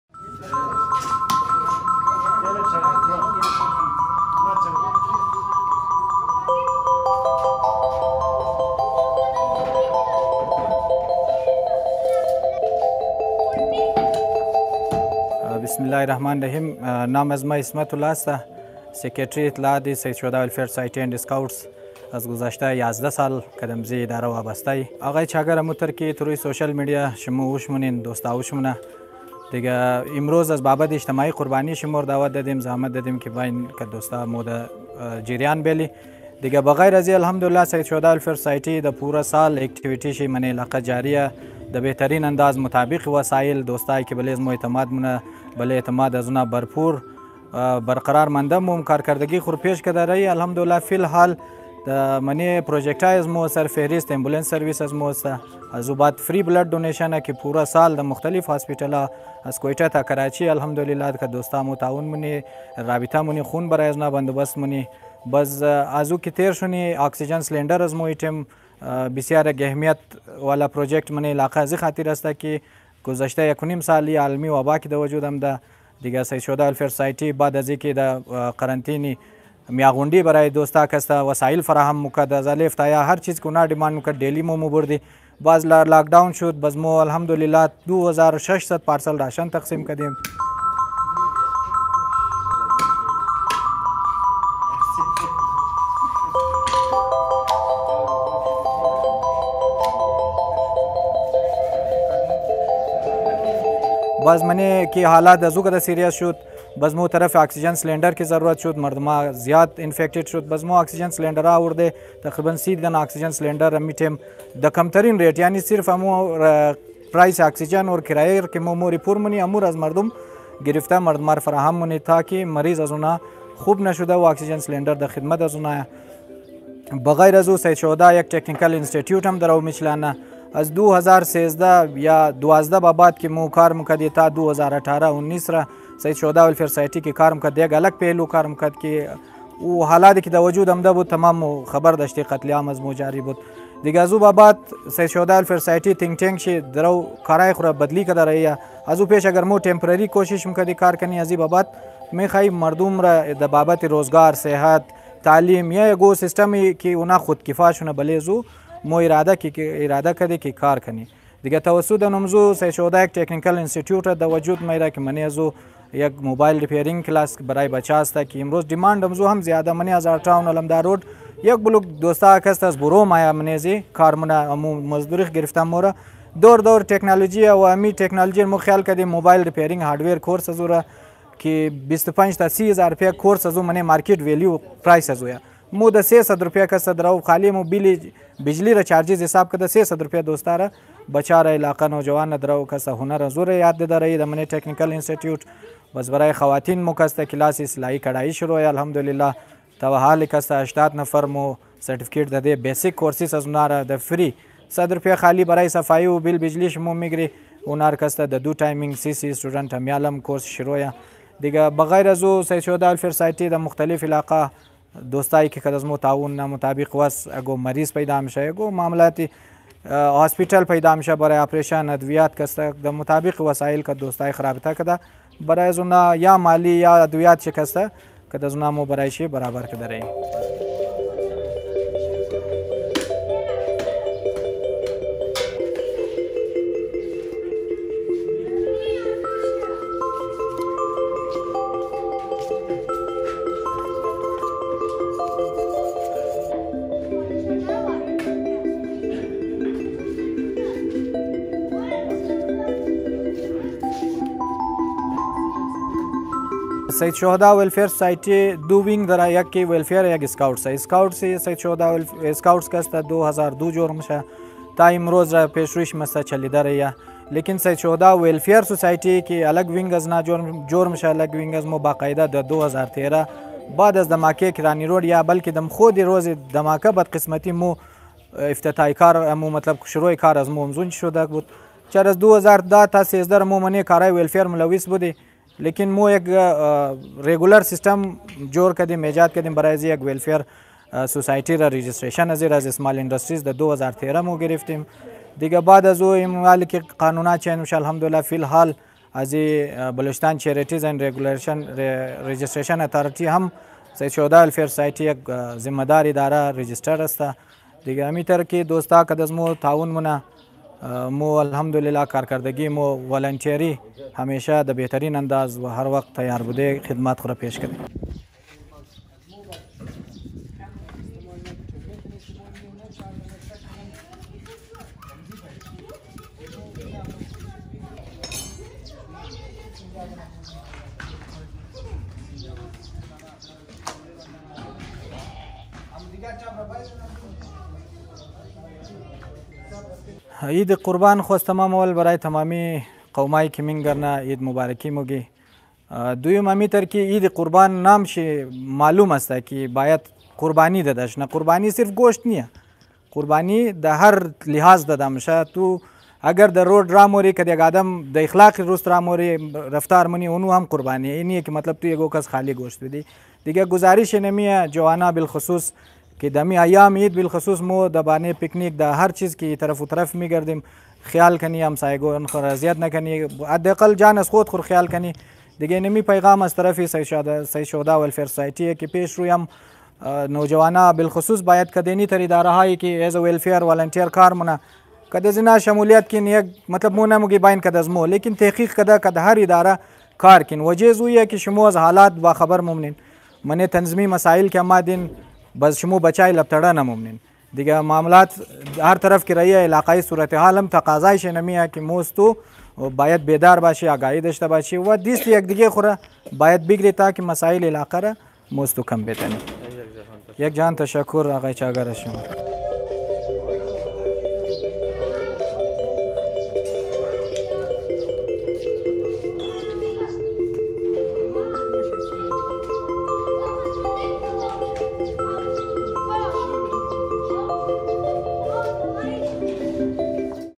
سیدالشہداء ویلفیر سوسائٹی رجسٹرڈ
اجتماعی قربانی برائے مستحقین | زیر اہتمام سیدالشہداء ویلفیر سوسائٹی رجسٹرڈ

VENUE | HAZARA TOWN QUETTA

Cameraman & Editor | Muhammad Hussain Chagar | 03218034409 | NOORBAN PRODUCTIONS |